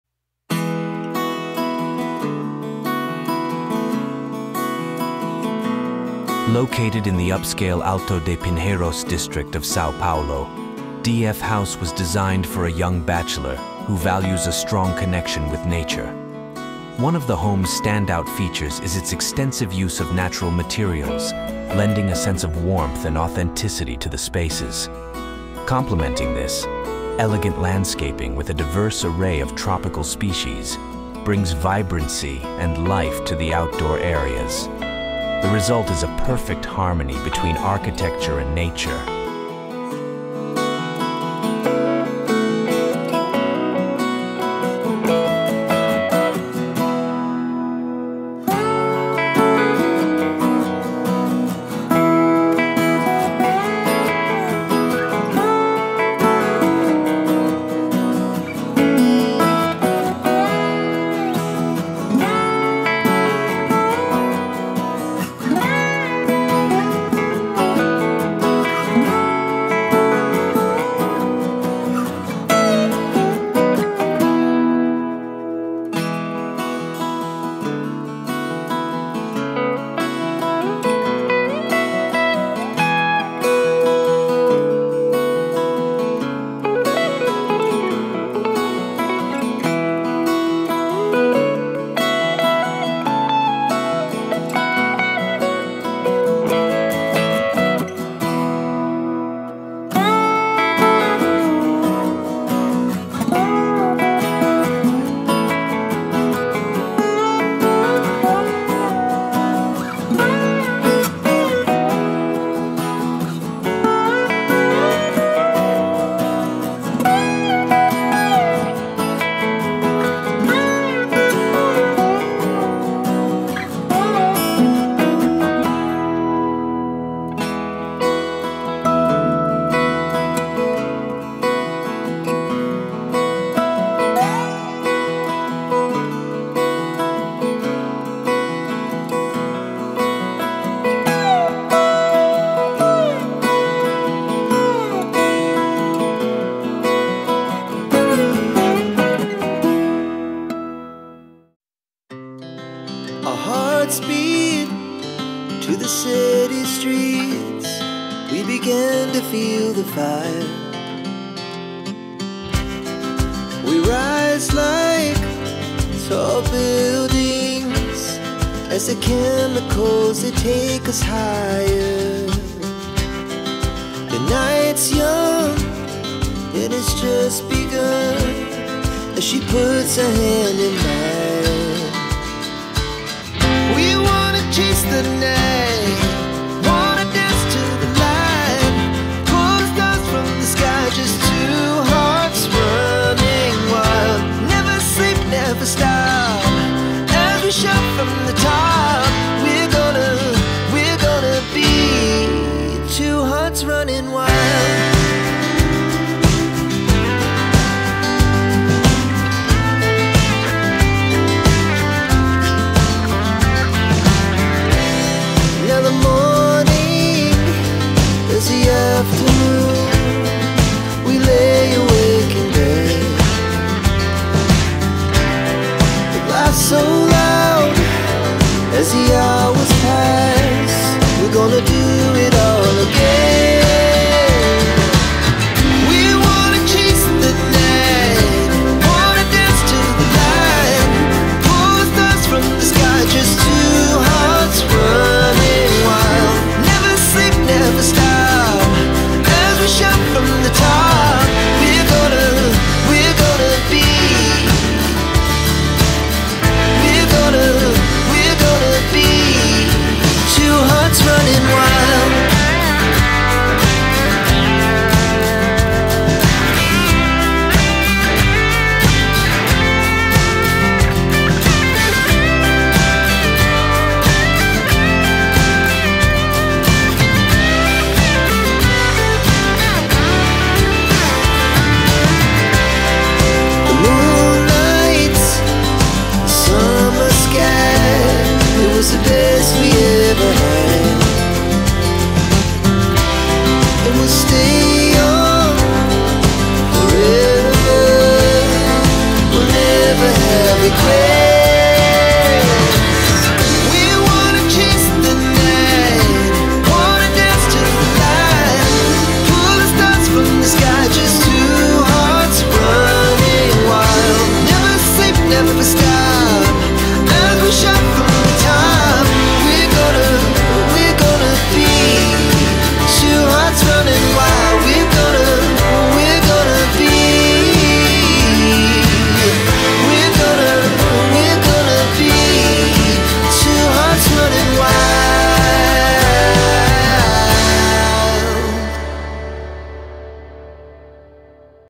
The architect brings a contemporary design that combines comfort and functionality to this residence
Located in Alto de Pinheiros, São Paulo, Casa DF underwent a renovation and interior design project led by the Rua 141 studio, showcasing elements of Brazilian design, natural textures, and tropical landscaping. With a 390m² plot and 200m² built across two floors, the house was redesigned to maximize natural light and ventilation, leveraging its large openings and spacious outdoor areas. The interior was strategically divided, with private spaces on the upper floor and social areas on the ground floor, offering a suite, office, bedrooms, living spaces, and services, all interconnected in a fluid and functional layout.

Tropical landscaping plays a crucial role in Casa DF, creating a harmonious transition between indoors and outdoors. Organic lines and tropical textures permeate the spaces, bringing life and a sense of integration with nature. The interior atmosphere combines industrial elements with a welcoming aura, highlighted by the contrast between raw steel and delicate natural straw, creating a unique and inviting visual harmony.

The careful selection of Brazilian-designed furniture and objects reinforces Casa DF's architectural identity. Handcrafted pieces and items from local designers add layers of personality and authenticity to each space, enriching the sensory experience for residents and visitors alike. The result is a house that not only shelters its inhabitants but also tells a visually and emotionally engaging story, where each element is an integral part of a cohesive and meaningful narrative.

The integration of architecture and interiors in Casa DF is exemplary, showcasing how attention to detail, harmony between materials, and balance between light, vegetation, and spaces can transform a residence into a true home. The house becomes more than a physical location; it becomes a living expression of the residents' lifestyle, personality, and identity, providing an environment that is both inspiring and welcoming.

Área: 200 m²
Ano: 2020
Fabricantes:  Blue Gardenia, Breton, Bruno de Carvalho, Carbono, Codexhome, Deca
Local: SÃO PAULO, BRASIL

0:00 -  DF House
2:45 - Living Room
4:00 - External Veranda
4:46 - Drawings